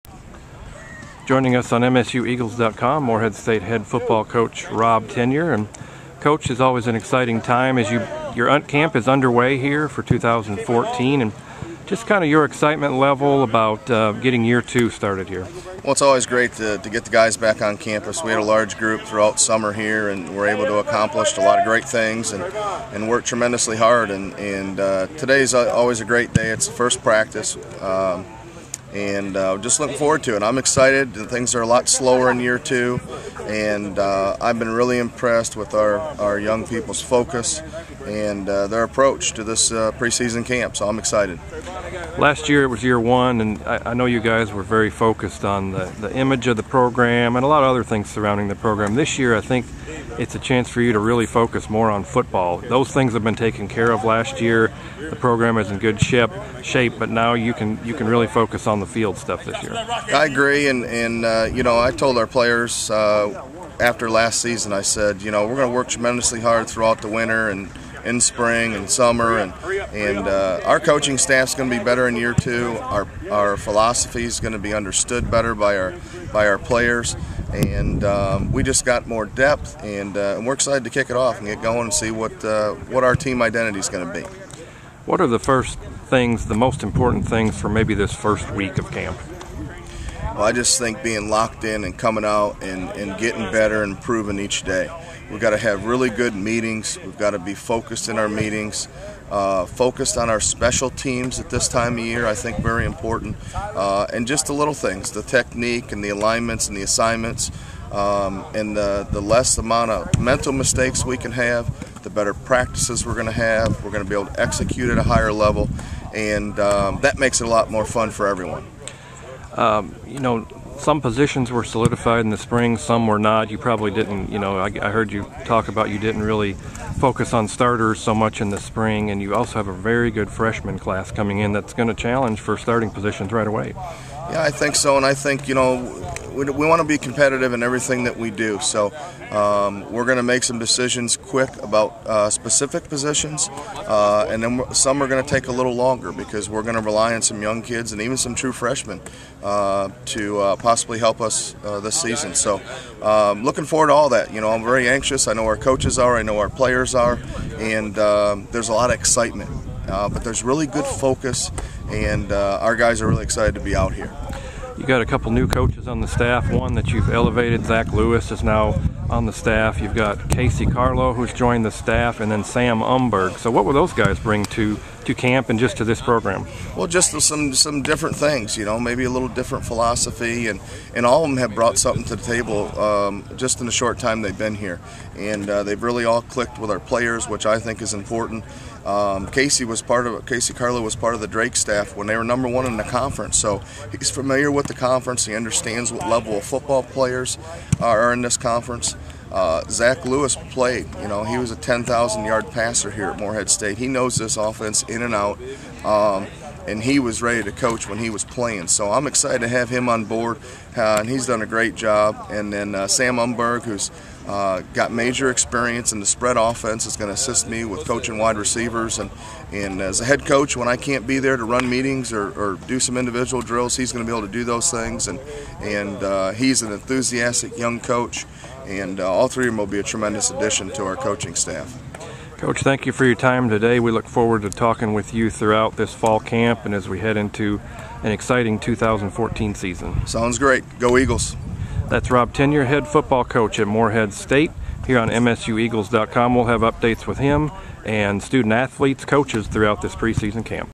Rob Tenyer Heading Into Preseason Camp
Head Football Coach Rob Tenyer chats with MSUEagles.com heading into 2014 preseason camp.